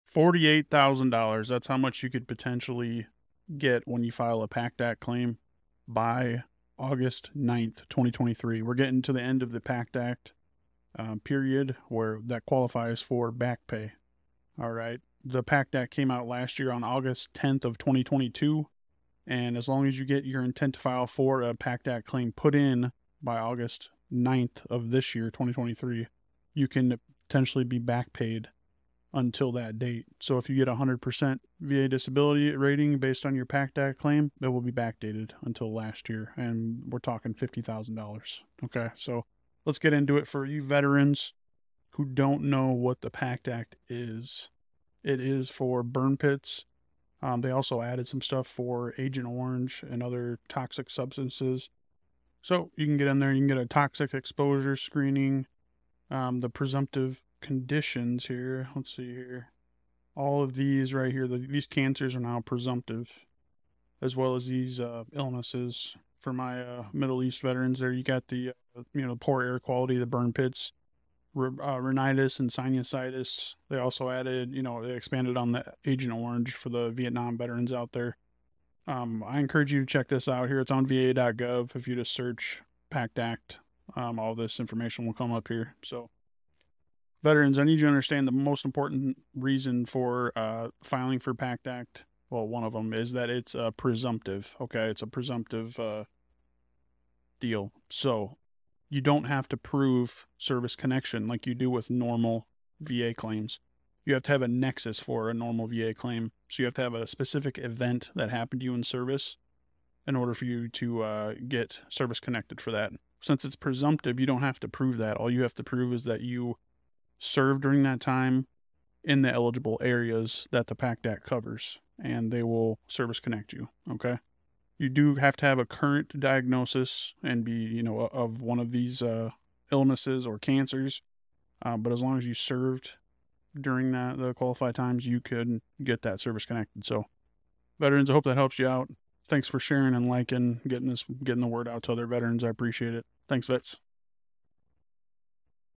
$50,000 VA Disability Backpay?
In this video we talk about how the PACT ACT timeline for backpay and how it is coming to a close. You will still be able to file a claim for the PACT ACT after August 9th, 2023 but you will not receive backpay from the date the act went in to effect. Don't leave that backpay on the table. Put your intent to file before 8/9/23 if you want that sweet backpay (if claim is granted service connection obviously)